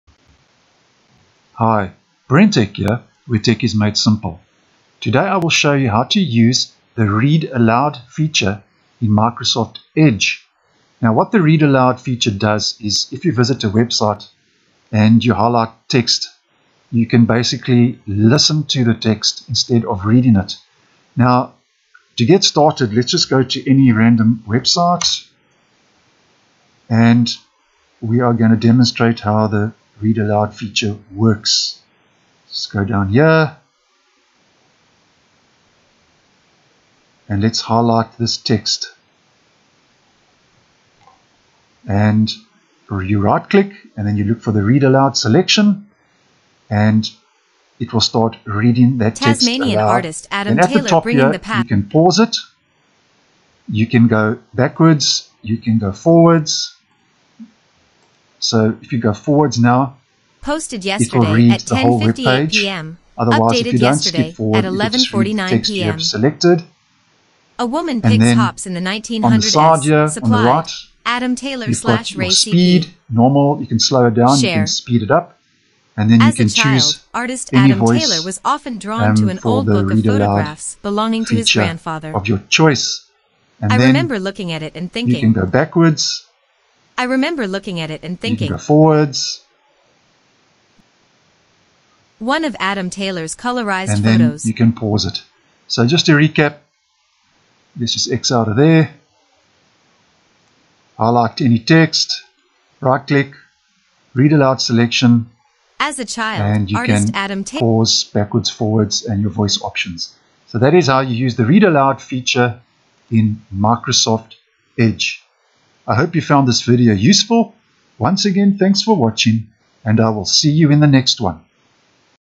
How to Use The Read Aloud Feature In Microsoft Edge Windows 10 20H2
I hope you enjoy this video on how to use the Read Aloud feature in Microsoft Edge.